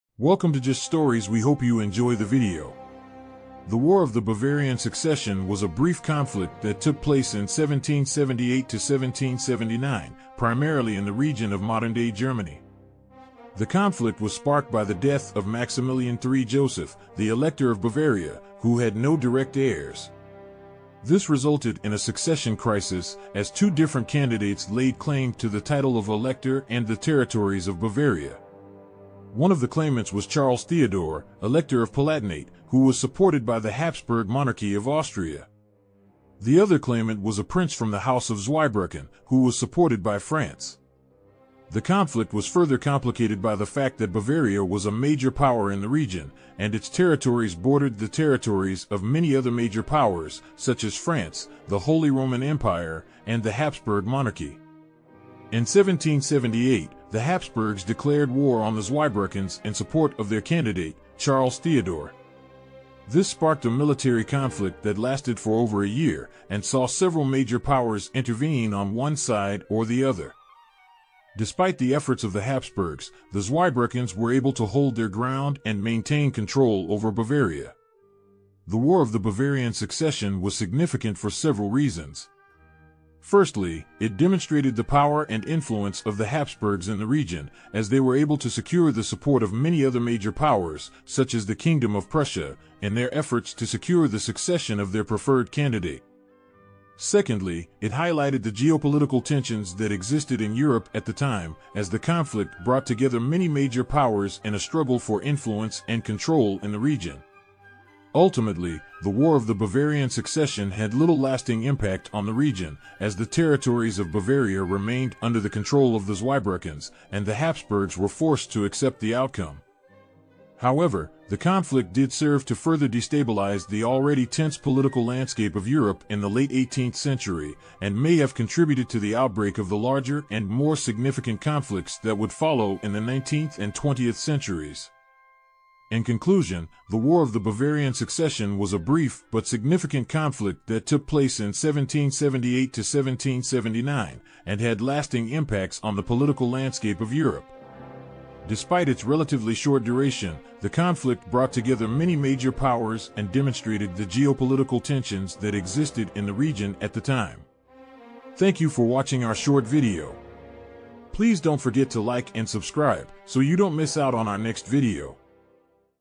JWS - SR05-EP16 - WAR OF THE BAVARIAN SUCCESSION 1778-79 CE
The War of the Bavarian Succession was a conflict between the Habsburgs of Austria and the French-backed prince of Zweibrücken over the succession of the Electorate of Bavaria. It took place in 1778-79 and was sparked by the death of Maximilian III Joseph, the Elector of Bavaria, with no direct heirs. The Habsburgs supported Charles Theodore, Elector of Palatinate, while France backed the prince of Zweibrücken. Despite the Habsburgs’ efforts, the Zweibrückens were able to maintain control over Bavaria. The war demonstrated the Habsburgs' power and influence, and highlighted the geopolitical tensions in Europe. It had little lasting impact but destabilized the political landscape and may have contributed to larger conflicts in the future.